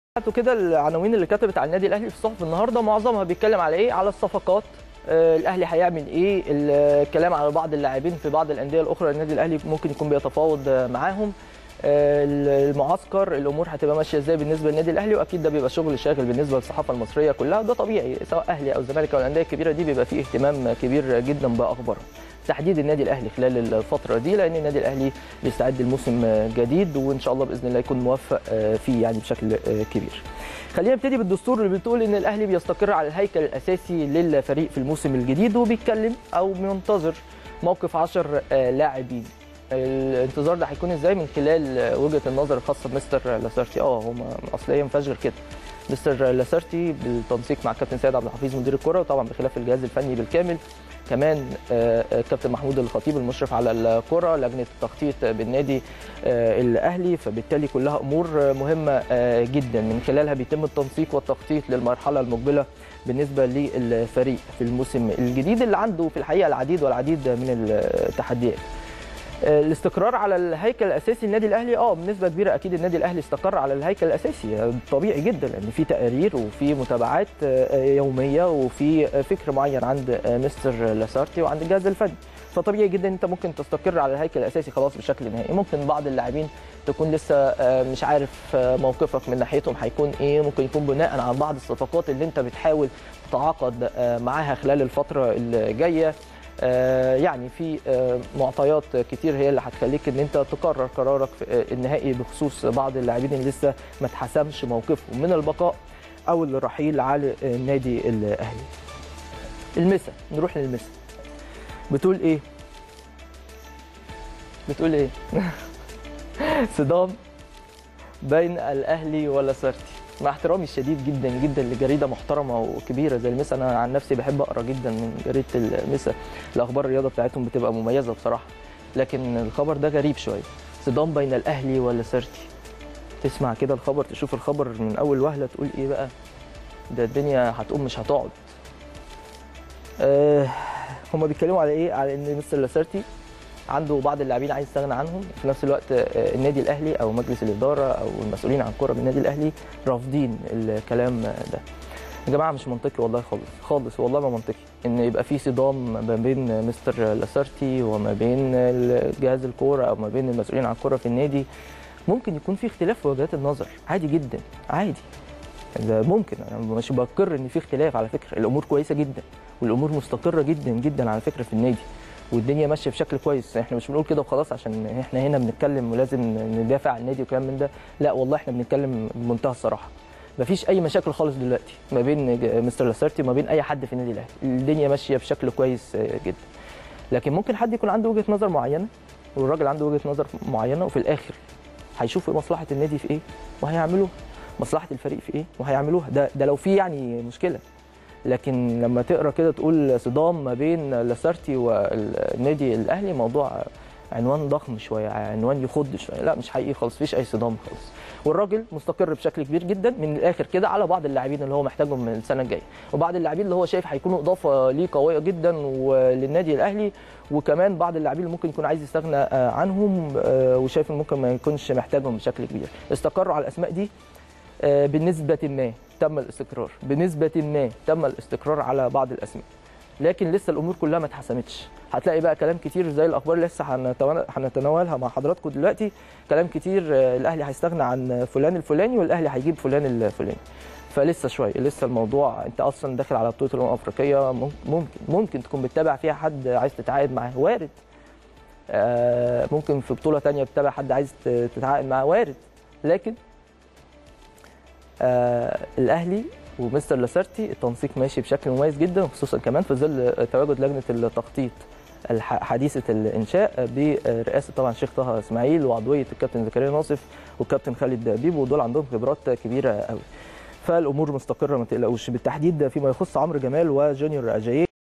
امير هشام يكشف حقيقة الاخبار المتعلقة برحيل بعض اللاعبين وموقف "لاسارتى"
التردد: 12341 أفقي.
القمر: نايل سات.
معامل الخطأ: 5/6.
معدل الترميز: 27500